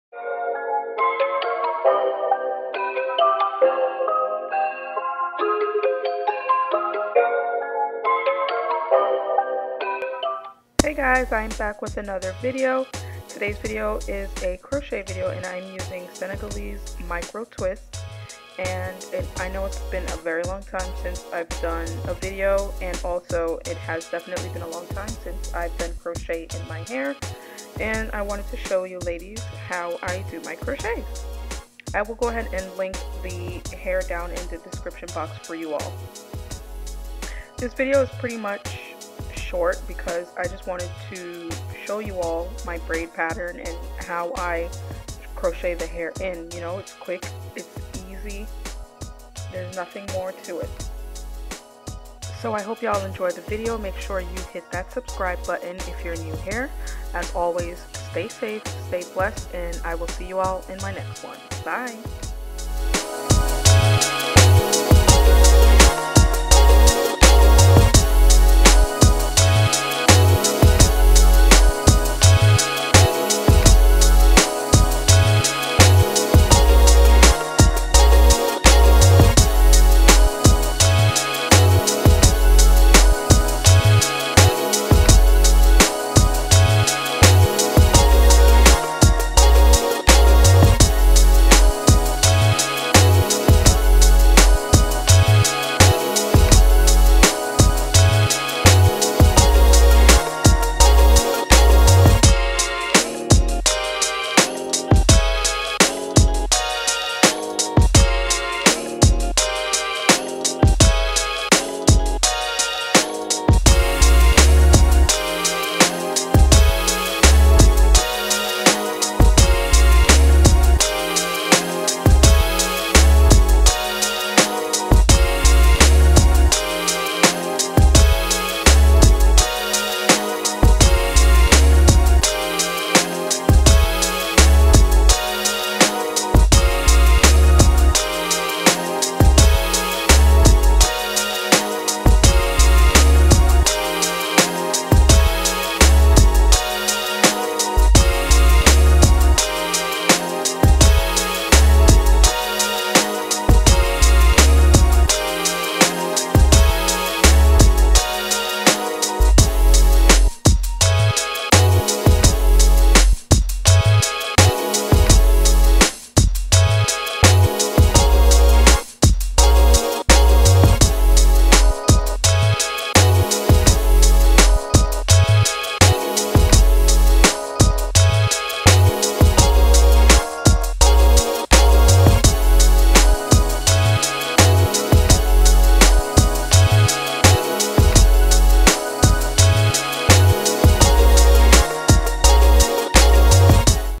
How To: Crochet Senegalese Micro Twists | Ombré | Quick and Easy
Hey guys! I am back with another video. So today's video i'm showing you guys how i crochet my Senegalese Ombré Twists in. It's a quick and easy tutorial. I do have other videos on my channel showing you how to crochet your hair. They are more detailed. This style took me 1 hour and 15 minutes to be exact. I was happy I did this in minimal time. But i hope this video was helpful and hope y'all enjoy. Stay safe, stay blessed and i will see y'all in my next one.

Hair details